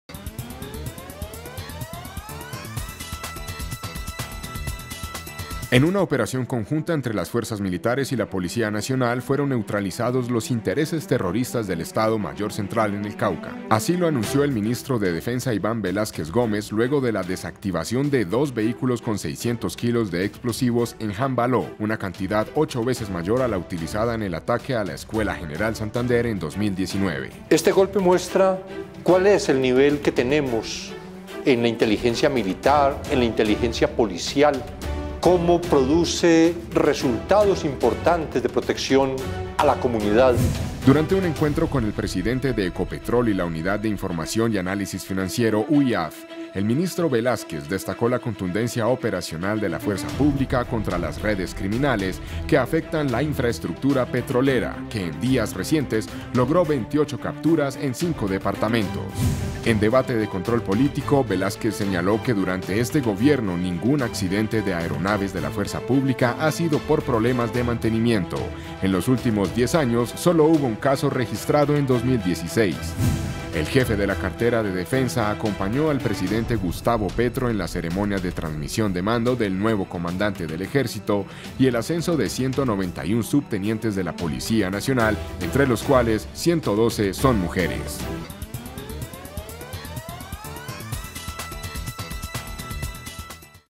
90Segundos| Fuerza Pública frustra acciones terroristas en el Cauca
✅Acciones de la Fuerza Pública para frustrar intenciones terroristas del EMC en Cauca

✅Balance del encuentro entre el ministro Iván Velásquez y Ricardo Roa
 presidente de Ecopetrol

✅Transmisión de mando del nuevo comandante del
Ejército Nacional

Estas y más noticias en 90Segundos